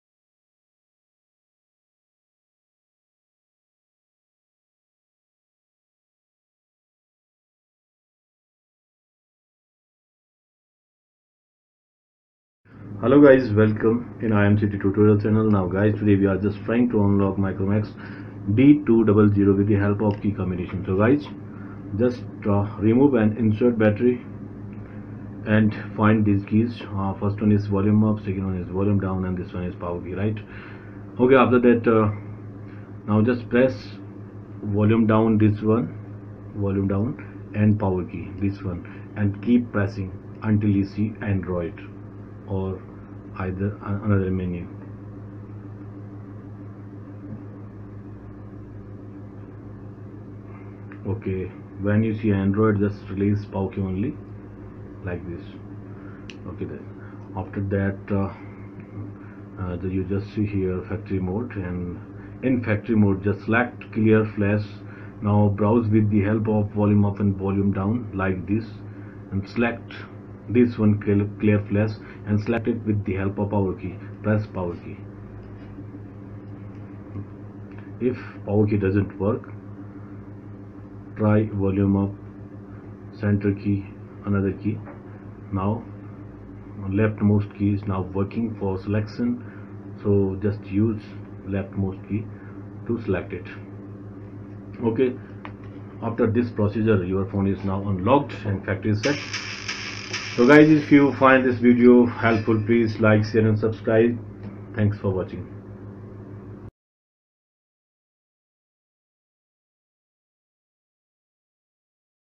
Micromax bolt D200 hard reset/ unlock/ factory reset tested 100%
How To Unlock Google Pattern Lock and Remove G mail Pattern Password of Micromax bolt D200  Android Phone.Micromax Mobile Phone Hard Reset Process Tips And Trick Learn In This Tutorial How To Hard Reset and Unlock Google Lock Without Any Software.
How To Hard Reset Micromax bolt D200 | Remove Pattern Lock | G-mail Password